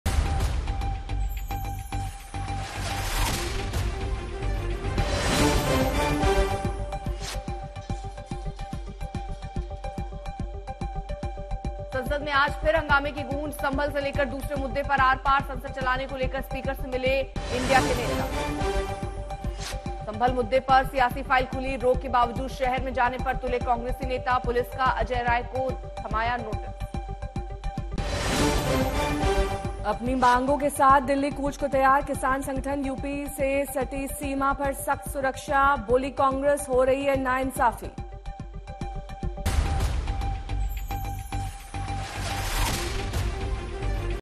Top Headlines Of The Day: Maharashtra CM Candidate | Ek Nath Shinde | Sambhal News | Fadnavis
महाराष्ट्र में नए सीएम का काउंटडाउन, बीजेपी विधायक दल की बैठक में हो सकता है ऐलान, शिंदे बोले- पीएम का निर्णय मंजूर

संभल पर लगातार सियासी सरगर्मी..आज कांग्रेस का संभल कूच, सुरक्षा इंतजाम सख्त, संसद में भी लगाातार मोर्चाबंदी

दिल्ली में चुनावी आहट से पहले केजरीवाल का अकेले लड़ने का एलान...तो रामलीला मैदान से कांग्रेस का शंखनाद- 8 दिसंबर से बीजेपी की परिवर्तन यात्रा

आबादी पर संघ प्रमुख मोहन भागवत का बड़ा बयान, बोले- बच्चे 3 अच्छे, कांग्रेस और ओवैसी का तंज- तो लागू करा दें स्कीम